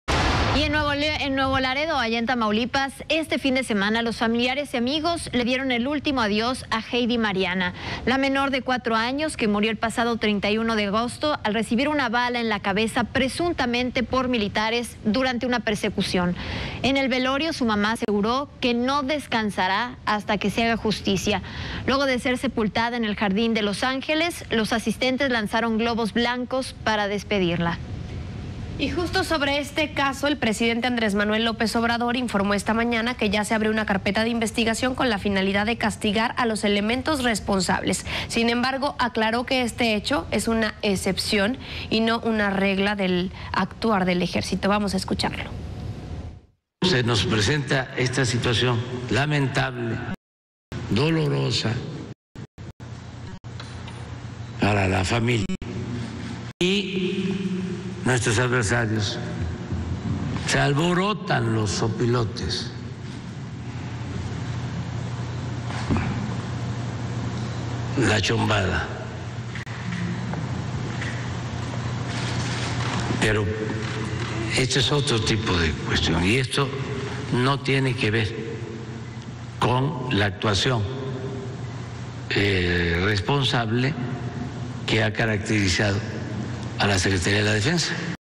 Asesinaron a menor; le dan último adiós
Familiares dan último adiós a Heidi Mariana de 4 años de edad; fue asesinada el pasado 31 de agosto tras recibir una bala en la cabeza presuntamente por militares en una persecución.